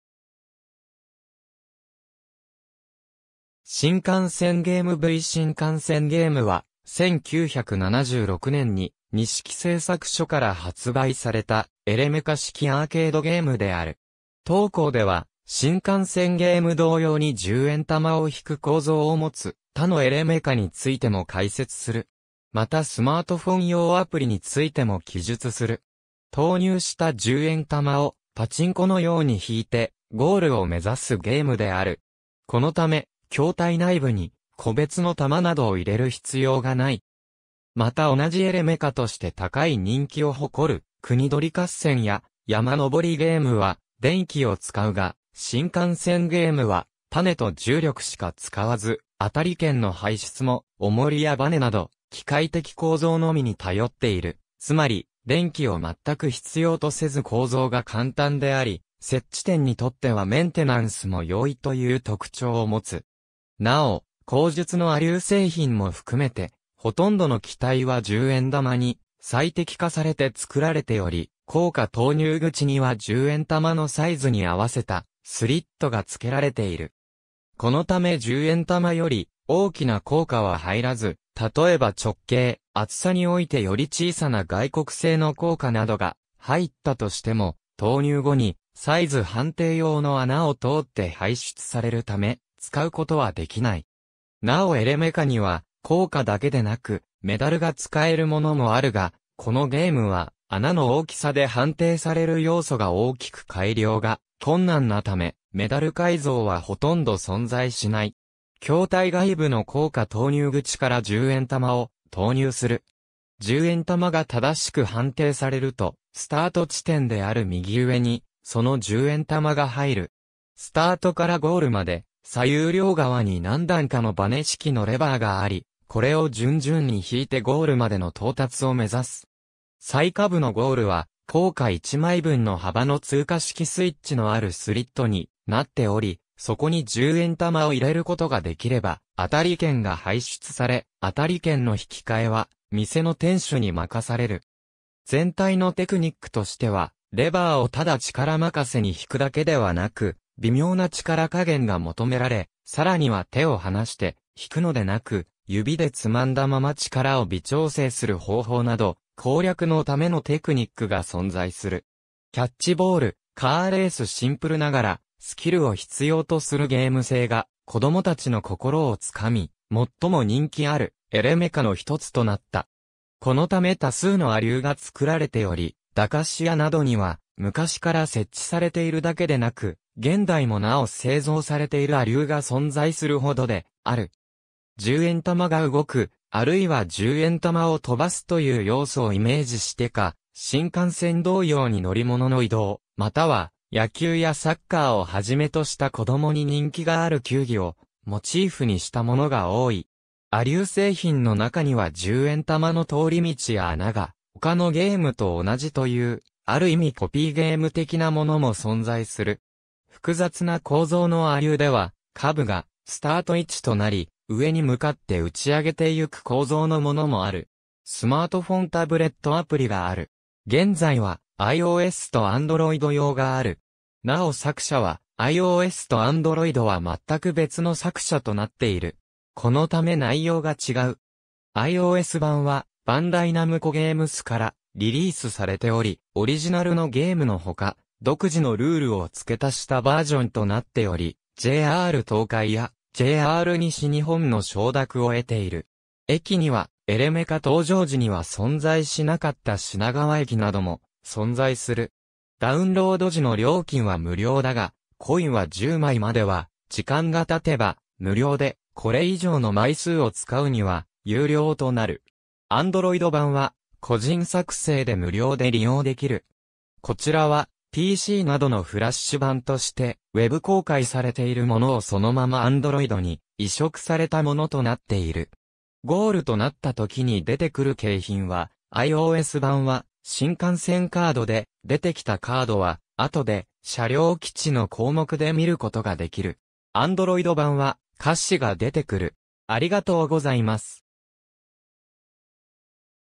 新幹線ゲーム
新幹線ゲームV 新幹線ゲーム（しんかんせん -）は、1976年にニシキ製作所から発売されたエレメカ式アーケードゲームである。
当稿では新幹線ゲーム同様に十円玉を弾く構造をもつ他のエレメカについても解説する。
またスマートフォン用アプリについても記述する。
投入した十円玉をパチンコの様に弾いて、ゴールを目指すゲームである。
このため筐体内部に個別の玉などを入れる必要がない。
また同じエレメカとして高い人気を誇る『国盗り合戦』や『山のぼりゲーム』は電気（ランプ）を使うが、新幹線ゲームはパネと重力しか使わず、当たり券の排出も重りやバネなど機械的構造のみに頼っている、つまり電気を全く必要とせず（明かりが点灯している様に見えるが、実は自然光を上から入れてアクリル板で照らしているだけである）構造が簡単であり、設置店にとってはメンテナンスも容易という特徴を持つ。
なお、後述の亜流製品も含めてほとんどの機体は十円玉に最適化されて作られており、硬貨投入口には十円玉のサイズに合わせたスリットが付けられている。
このため十円玉より大きな硬貨は入らず、たとえば直径・厚さにおいてより小さな外国製の硬貨などが入ったとしても、投入後にサイズ判定用の穴を通って排出されるため、使うことは出来ない。
なおエレメカには硬貨だけでなくメダルが使えるものもあるが、このゲームは穴の大きさで判定される要素が大きく改良が困難なため、メダル改造は殆ど存在しない。
筐体外部の硬貨投入口から十円玉を投入する。
十円玉が正しく判定されると、スタート地点である右上にその十円玉が入る。
スタートからゴールまで左右両側に何段かのバネ式のレバーがあり、これを順々に弾いてゴールまでの到達を目指す。
最下部のゴールは硬貨一枚分の幅の通過式スイッチのあるスリットになっており、そこに十円玉を入れることができれば、当たり券が排出され、当たり券の引き換えは店の店主に任される。
全体のテクニックとしては、レバーをただ力任せに弾くだけではなく、微妙な力加減が求められ、さらには手を離して弾くのでなく、指でつまんだまま力を微調整する方法など、攻略のためのテクニックが存在する。
『キャッチボール』 『カーレース』 シンプルながらスキル（腕前）を必要とするゲーム性が子供達の心をつかみ（当時熱中したプレイヤーの中には、100%必ずゴールまで持って行ける実力を身に付けた者もいた）、最も人気あるエレメカの一つとなった。
このため多数の亜流が作られており、駄菓子屋などには昔から設置されているだけでなく、現代もなお製造されている亜流が存在する程である。
十円玉が動く、あるいは十円玉を飛ばすという要素をイメージしてか、新幹線同様に乗り物の移動、または野球やサッカーをはじめとした子供に人気がある球技をモチーフにしたものが多い。
亜流製品の中には十円玉の通り道や穴が他のゲームと同じという、ある意味コピーゲーム的なものも存在する。
複雑な構造（透明なアクリル板で手前と奥を仕切り、ゲームフィールドを2倍使えるなど）の亜流では、下部がスタート位置となり、上に向かって打ち上げてゆく構造のものもある。
スマートフォン・タブレットアプリがある。
現在はiOS（iPhone・iPad）とAndroid用がある。
なお作者はiOSとAndroidは全く別の作者となっている。
このため内容が違う。
iOS版はバンダイナムコゲームスからリリースされており、オリジナルのゲームの他、独自のルールを付け足したバージョンとなっており、JR東海やJR西日本の承諾を得ている。
駅にはエレメカ登場時には存在しなかった品川駅なども存在する。
ダウンロード時の料金は無料だが、コインは10枚までは時間が経てば無料で、これ以上の枚数を使うには有料となる。
Android版は個人作成で無料で利用できる。
こちらはPCなどのFlash版としてWEB公開されているものをそのままAndroidに移植されたものとなっている。
ゴールとなったときに出てくる景品はiOS版は新幹線カードで、出てきたカードは後で車両基地の項目で見ることが出来る。
Android版は菓子が出てくる。